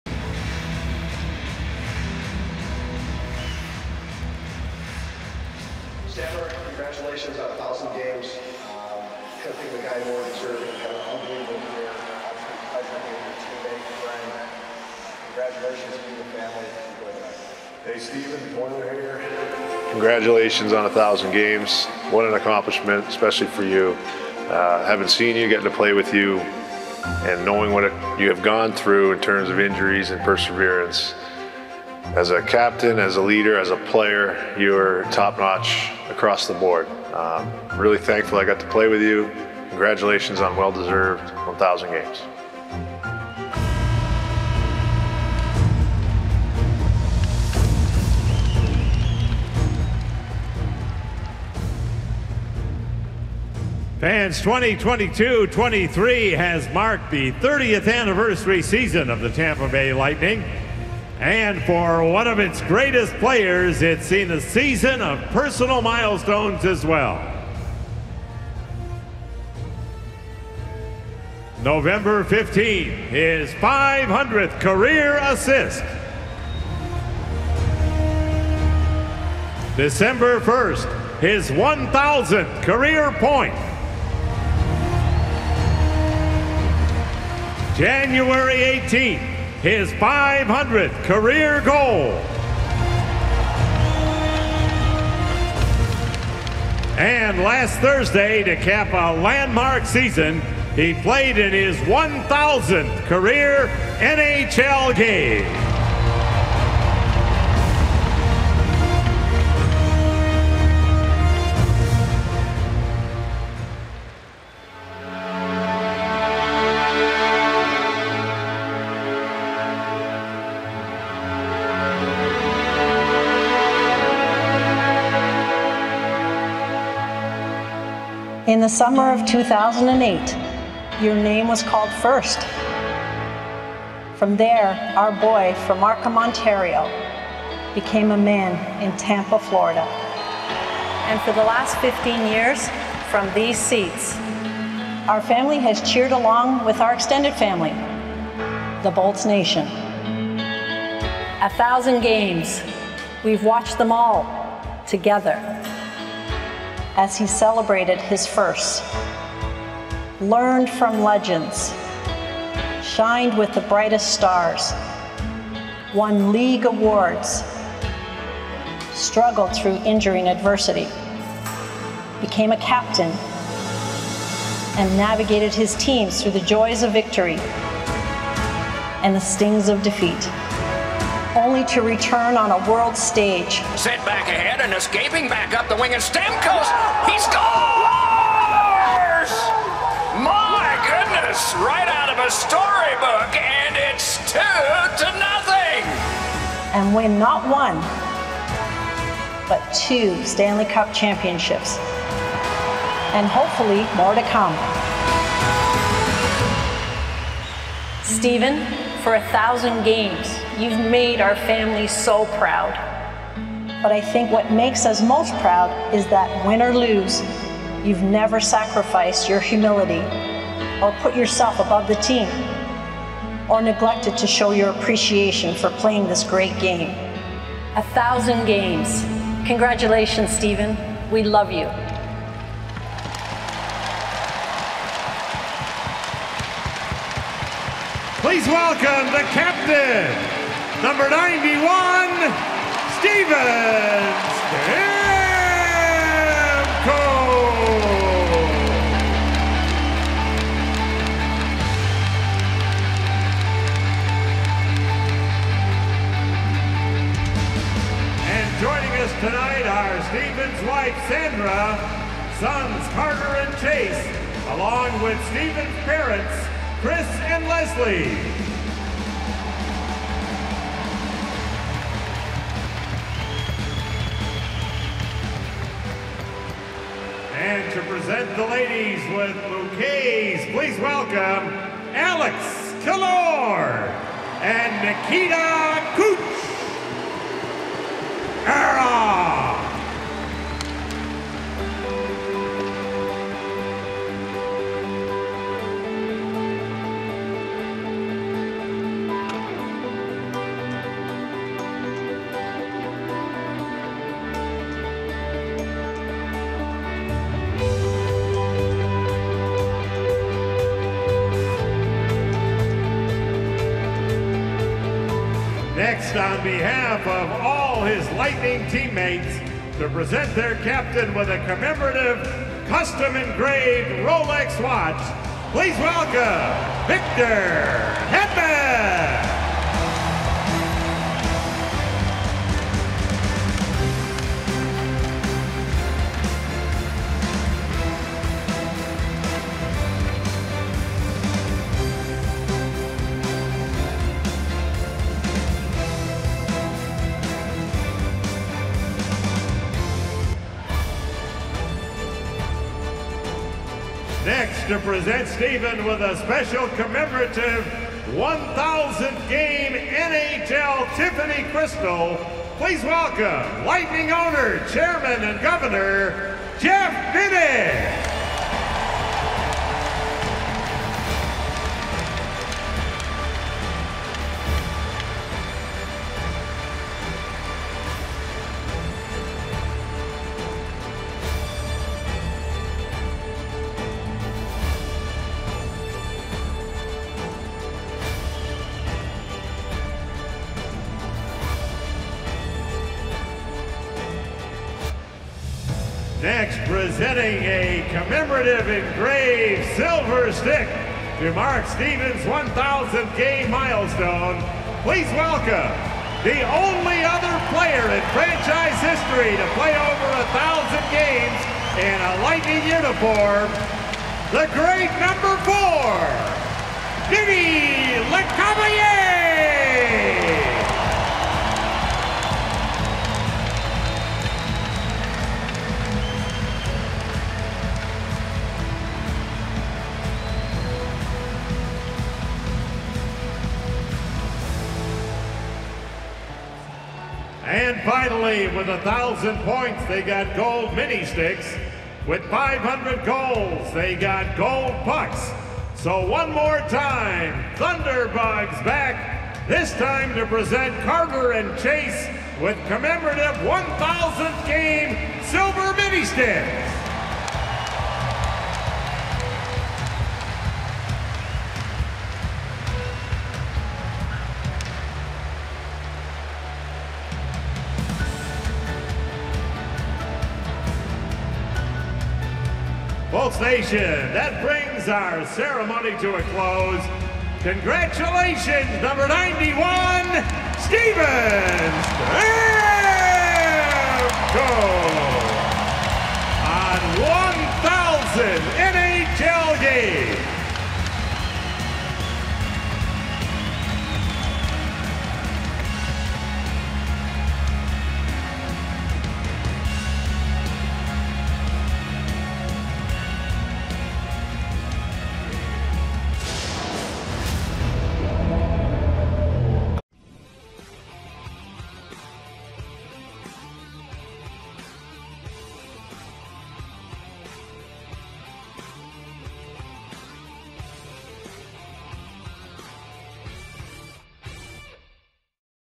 Lightning Honour Steven Stamkos Reaching 1,000-Game Milestone With Pre-Game Ceremony
Watch as the Tampa Bay Lightning honour captain and two-time Stanley Cup champion Steven Stamkos with a pre-game ceremony and gifts for playing in his 1,000th NHL game.